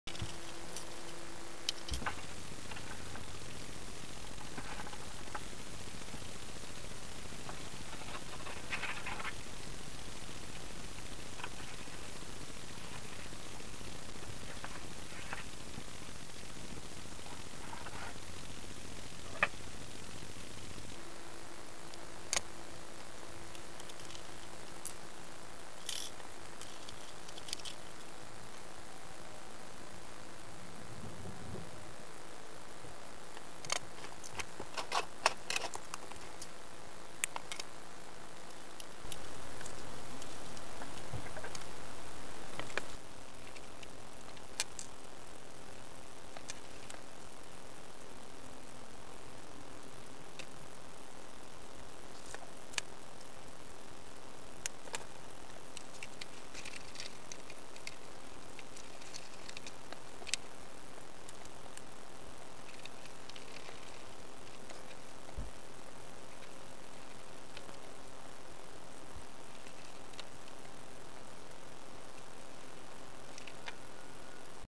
54 Tragfläche Teil 24: Einbau Anlenkung
Einbau der Anlenkung der Querruder.